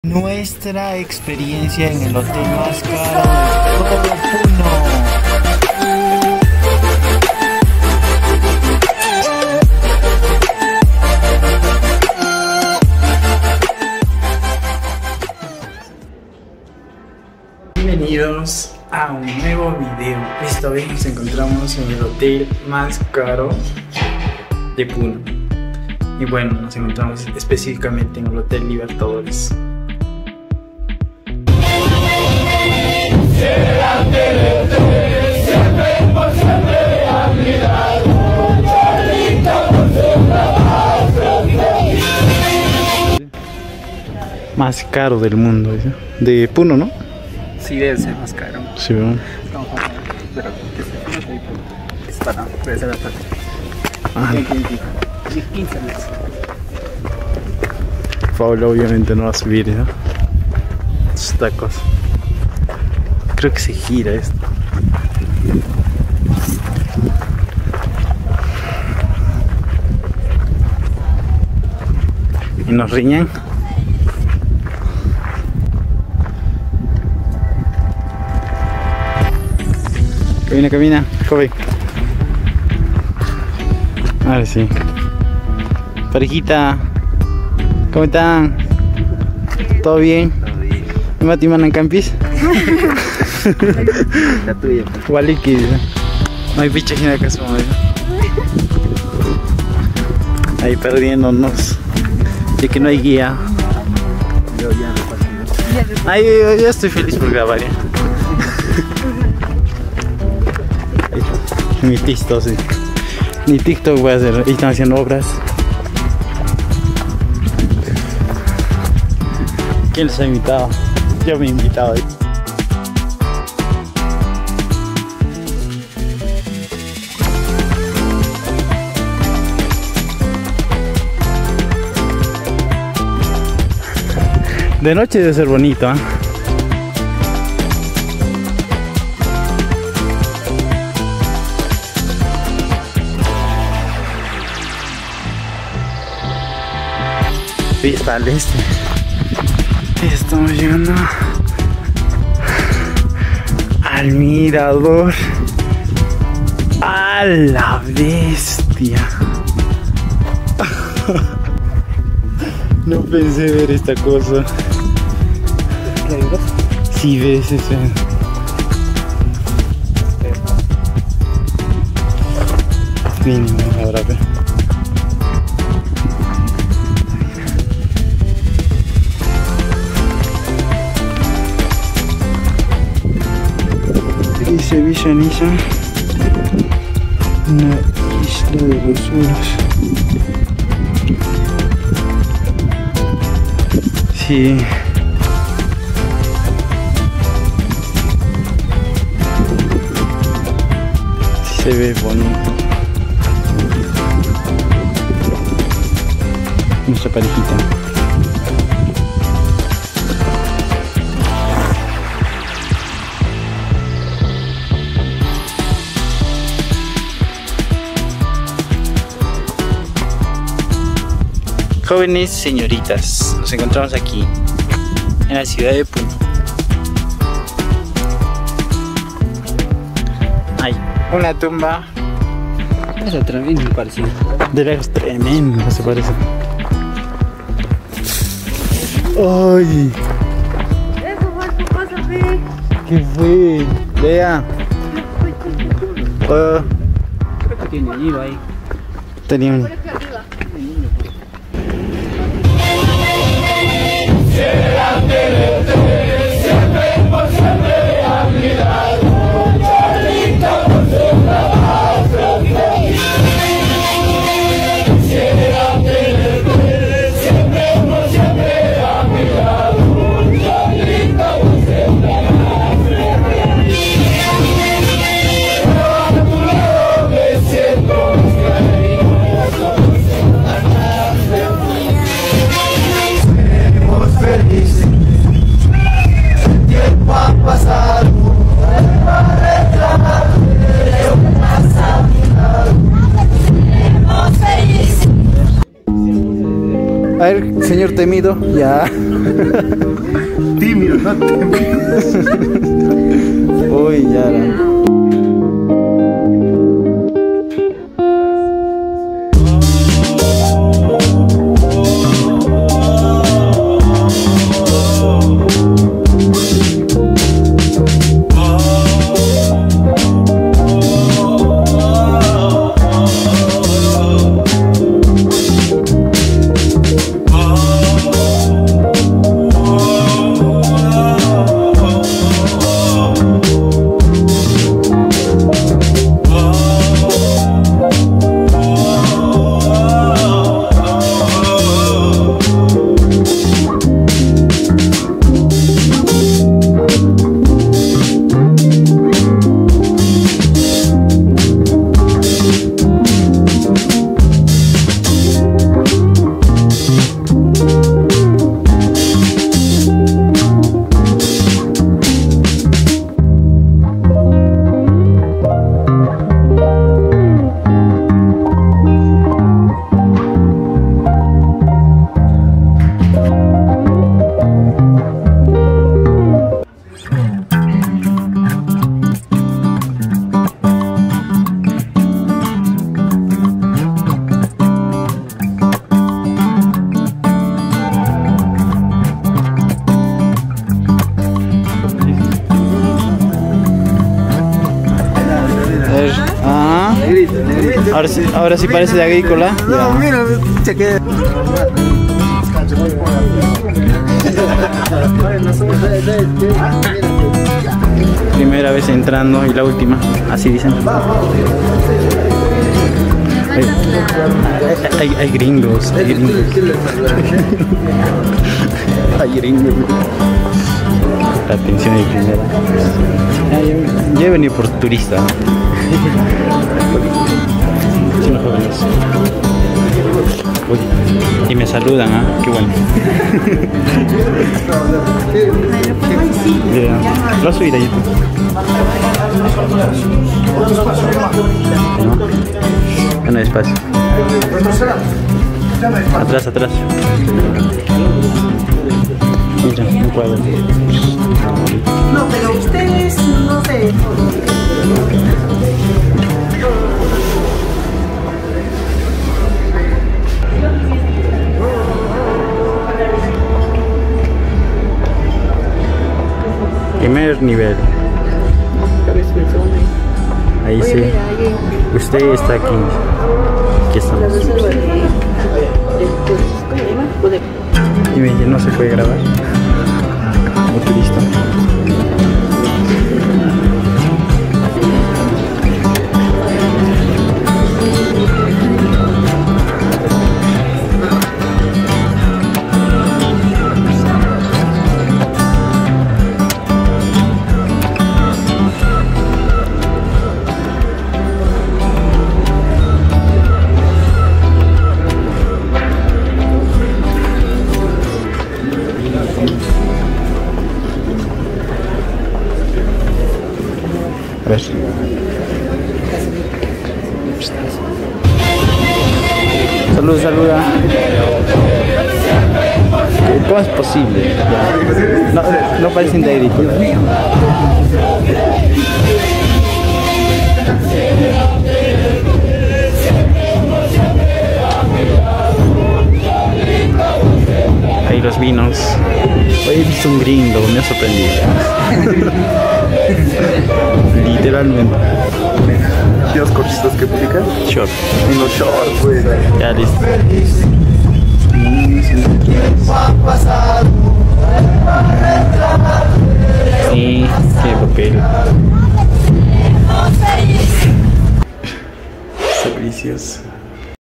Descubriendo el Hotel Más Exclusivo de Puno: Nuestra Experiencia de Lujo
¡Bienvenidos a mi canal! En este episodio especial, los llevo conmigo a descubrir el hotel más exclusivo y lujoso de Puno. Prepárense para una experiencia única mientras exploramos cada rincón de este oasis de confort y elegancia en el corazón de esta encantadora ciudad peruana.

Desde el momento en que llegamos, quedamos impresionados por la majestuosidad de sus instalaciones y la atención meticulosa al detalle. Les mostraré las impresionantes vistas del lago Titicaca y los Andes que este hotel ofrece, así como las comodidades de clase mundial que hacen de cada momento una verdadera delicia.

Acompáñenme mientras descubrimos los secretos y las sorpresas que este hotel tiene reservados para nosotros. Desde la gastronomía exquisita hasta las experiencias de bienestar y relajación, les mostraré por qué este lugar es más que un simple alojamiento: es una experiencia de lujo incomparable.

No se pierdan este viaje de lujo y sofisticación en Puno. ¡Nos vemos en el próximo destino!